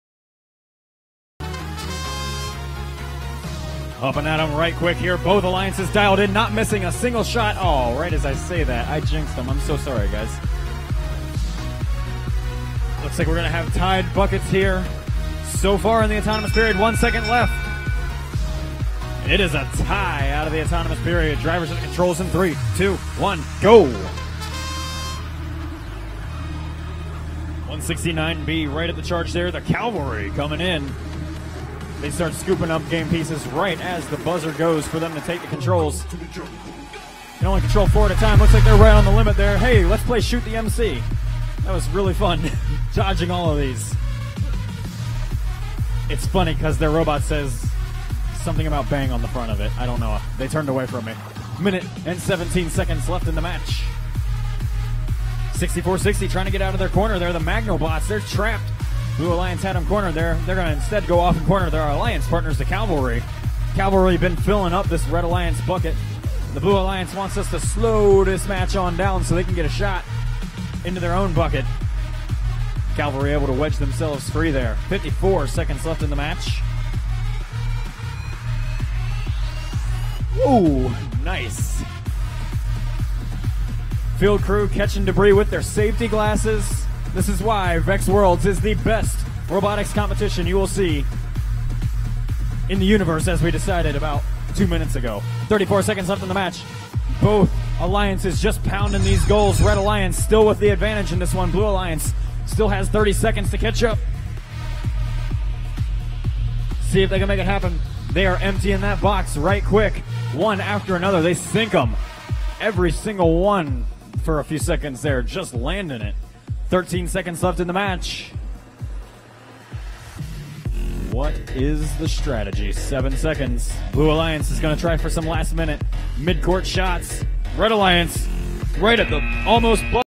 VEX Worlds 2016 - VRC High School - Arts - Qual 135 (169B 6460) 226 vs 168 (4393R 3818A)
VEX Worlds 2016 - VRC High School - Arts - Qual 135

Red - Score 226:
169B - The Cavalry (Haverford School Robotics, Haverford, Pennsylvania, United States)
||  Arts Rank:7 . Wins:9 . Loses:1  . WP:18 . SP:1482

6460 - MAGNOBOTS (magnolia science academy 1, reseda, California, United States)
||  Arts Rank:81 . Wins:3 . Loses:7  . WP:6 . SP:1580

Blue - Score 168:
4393R - Penguins B (Maine School of Science and Mathematics, Limestone, Maine, United States)
||  Arts Rank:75 . Wins:4 . Loses:6  . WP:8 . SP:1234

3818A - ISManila (International School Manila, Manila, Philippines)
||  Arts Rank:91 . Wins:2 . Loses:8  . WP:4 . SP:1635